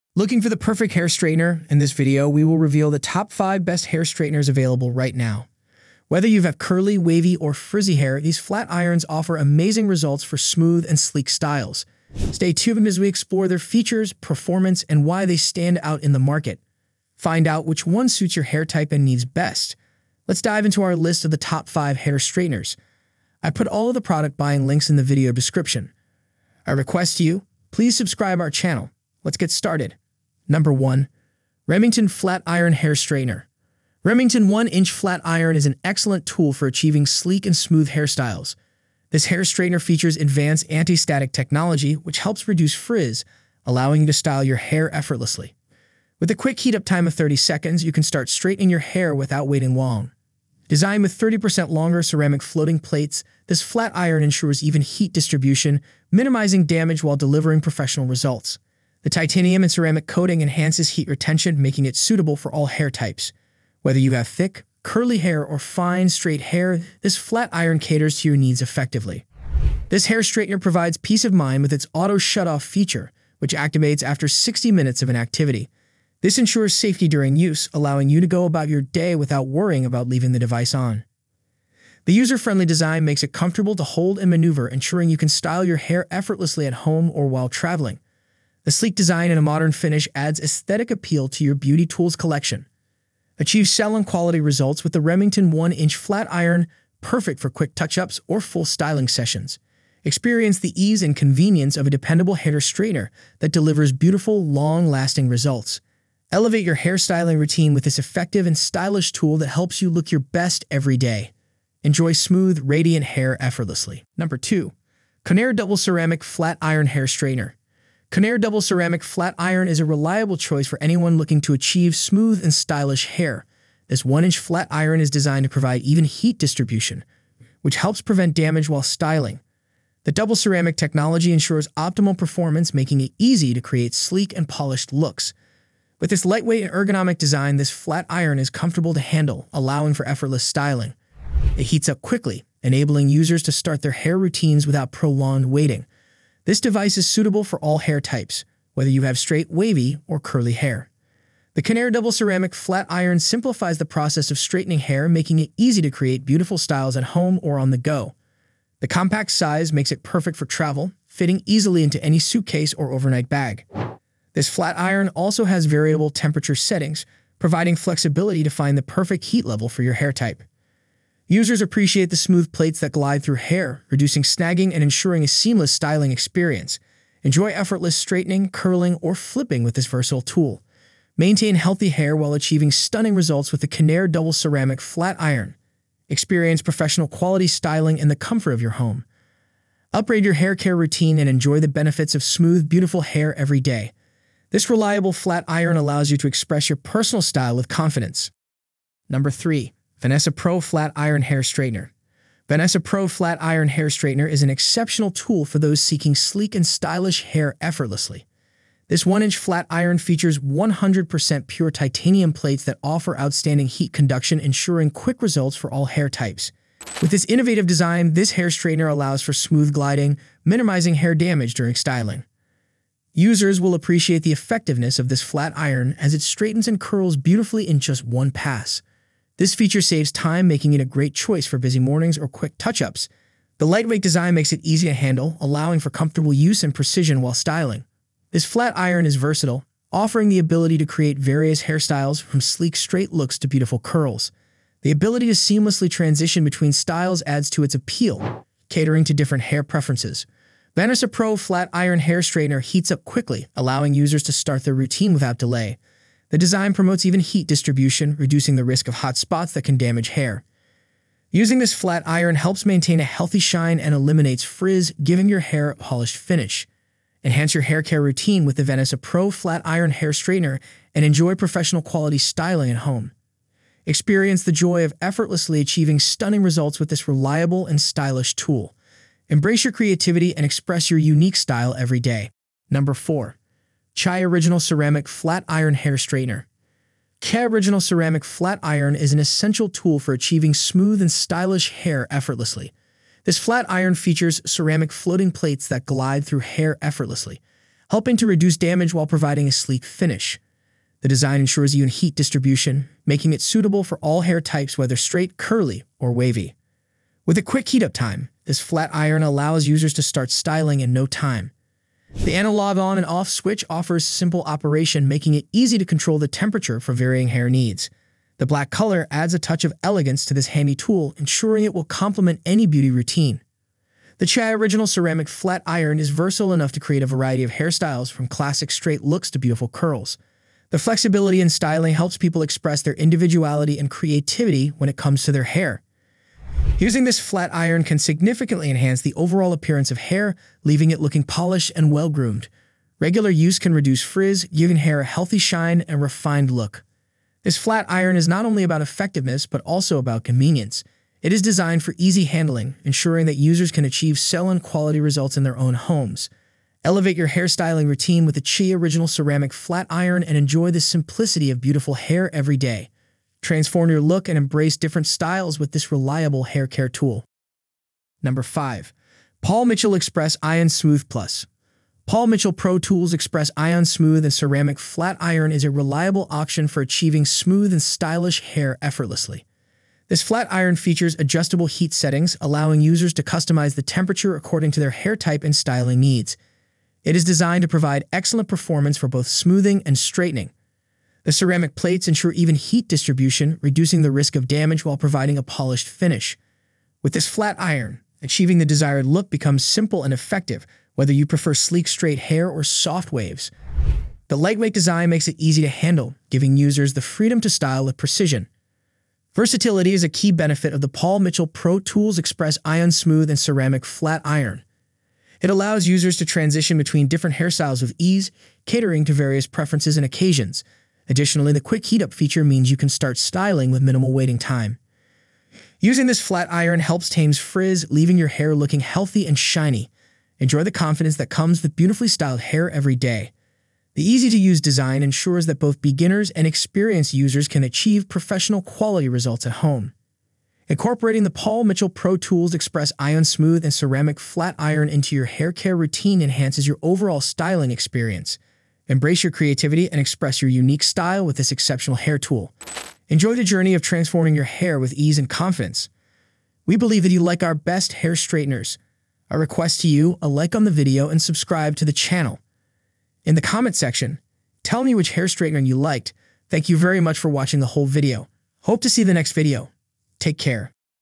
Top 5 Best Hair Straighteners and Flat Irons for All Hair 2025
👇 Top 5 Best Hair Straighteners 👇
► 1. Remington Flat Iron Hair Straightener

► 2. Conair Double Ceramic Flat Iron Hair Straightener

► 3. VANESSA PRO Flat Iron Hair Straightener

► 4. CHI Original Ceramic Flat Iron Hair Straightener

► 5. Paul Mitchell Express Ion Smooth+

If you are searching for the best hair straightener, this video covers the top five options available. These straighteners work well for various hair types, from curly and thick to fine and frizzy. Whether you are looking to achieve sleek, smooth hair or eliminate frizz, these straighteners are designed to give professional results at home. In this video, we will share the top choices to help you find the perfect match for your styling needs. From budget-friendly models to high-end options, each of these straighteners offers excellent performance, helping you achieve salon-quality hair.

Choosing the right straightener is essential for maintaining healthy hair and achieving the style you want. These five picks stand out in the market for their effectiveness, ease of use, and reliability. Watch the video to find out which straightener suits your needs best and start enjoying sleek, shiny, and manageable hair every day.